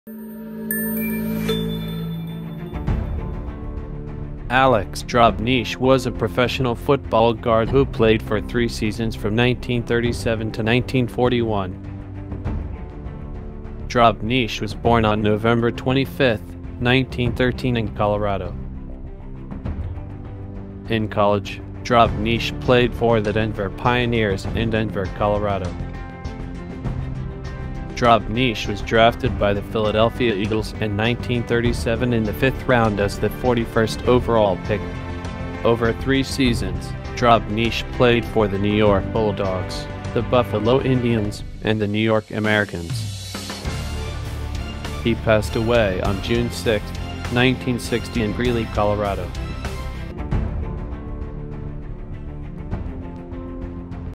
Alex Drobnitch: Football Guard
Alex Drobnitch was a professional football guard who played for 3 seasons from 1937 to 1941.

In college, Drobnitch played for the Denver Pioneers in Denver Colorado.

Professionally, Drobnitch played for the New York Bulldogs, the Buffalo Indians, and the New York Americans.